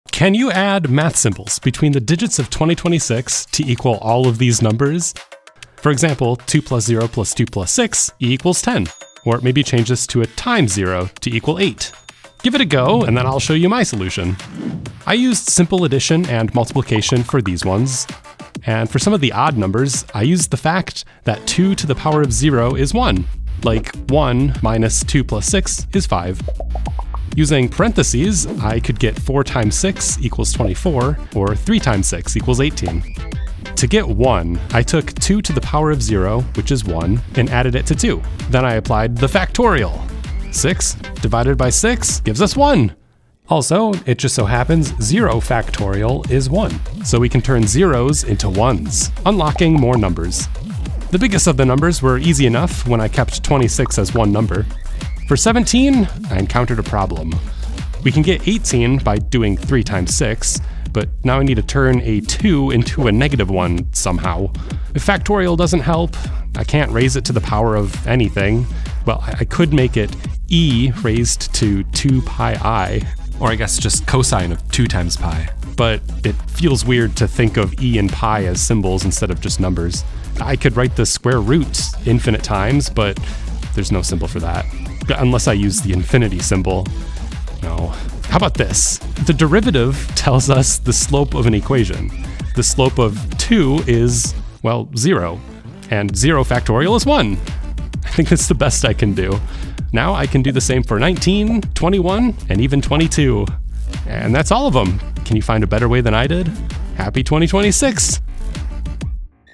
The 2026 Problem maths mathpuzzle #2026
I attempt this challenge every year on my own. This year I wanted to share the puzzle with you!